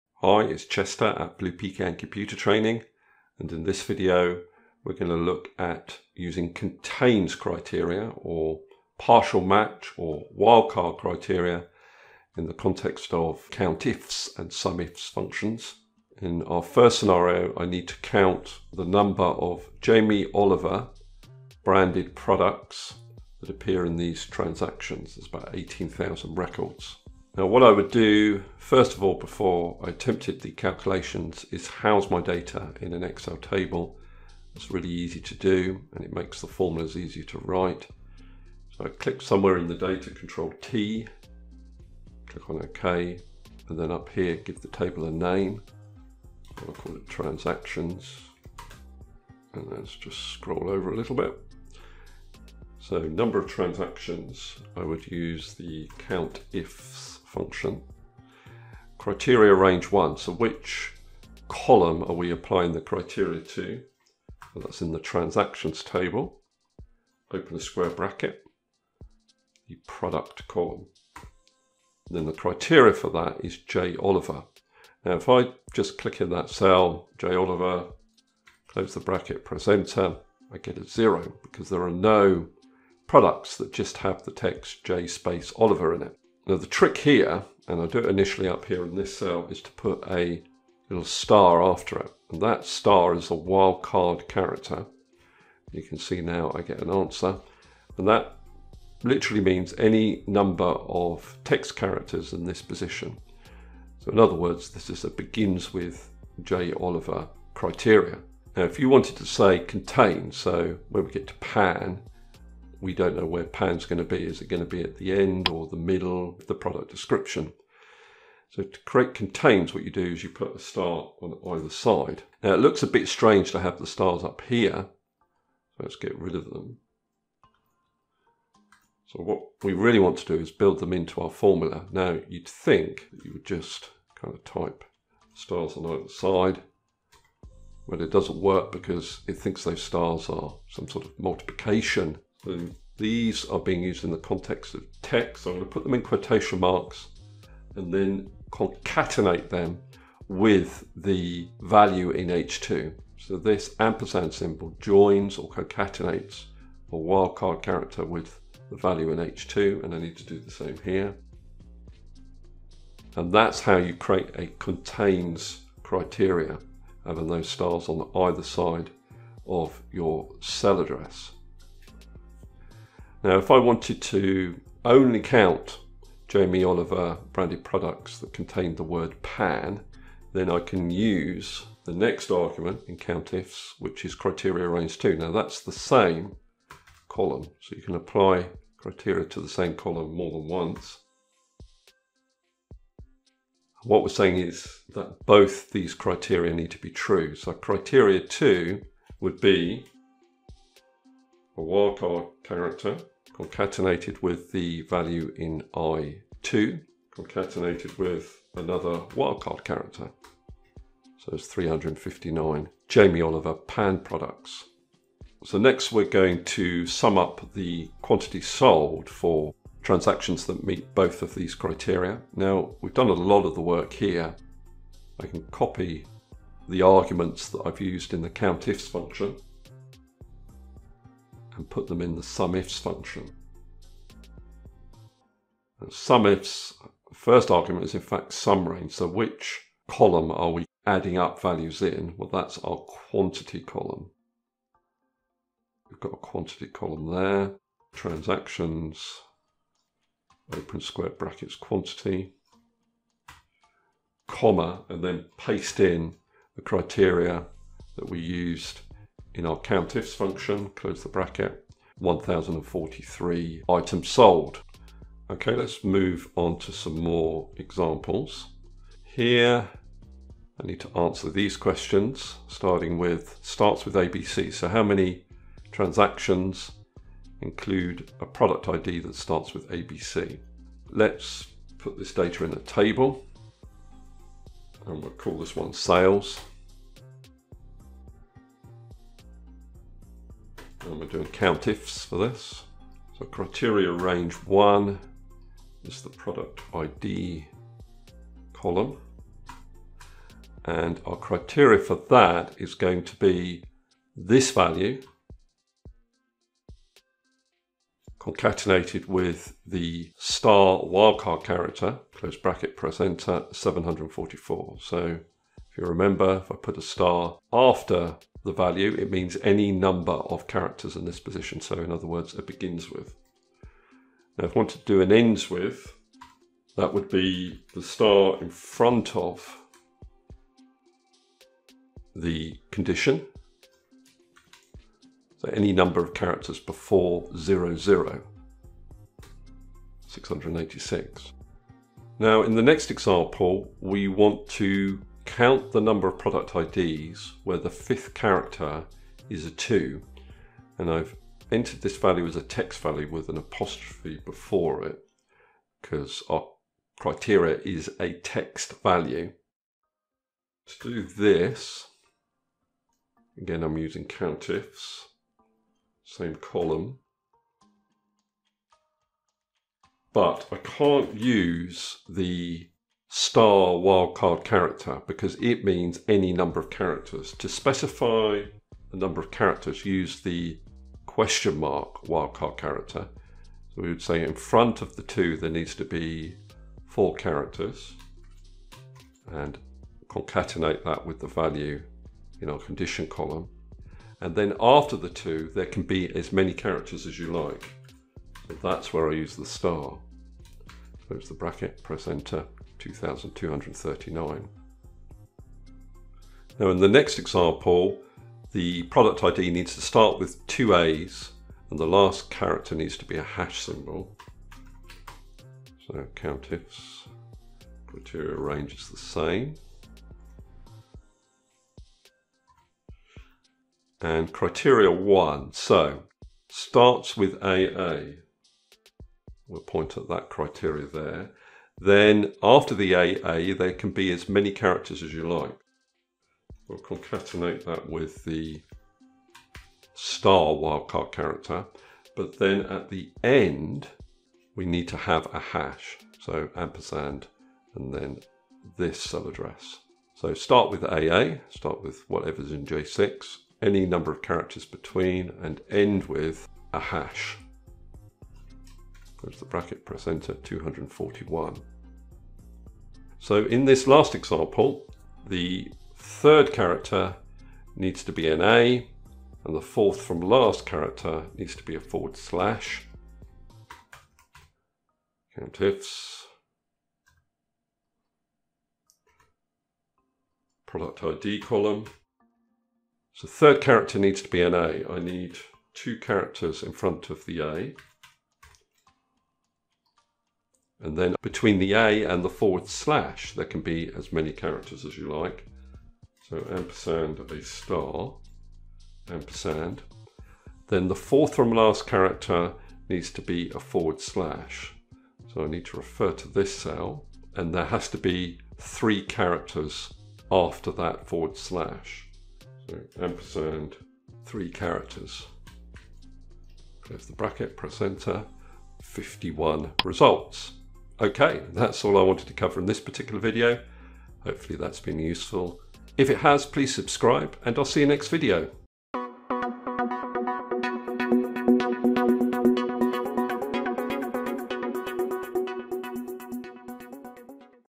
SUMIFS & COUNTIFS Partial Match | Part of Cell Contains Text | Using * and ? Wildcard Characters
In this video I demonstrate how to perform partial match SUMIFS and COUNTIFS calculations using the wildcard characters * and ?

1) How to perform a contains text COUNTIFS or SUMIFS calculation with one or more criteria
2) How to perform a begins with or ends with text, COUNTIFS or SUMIFS calculation
3) How to specify the position of the character in your criteria, eg "/" must be the fourth from last character or "#" must be the second character.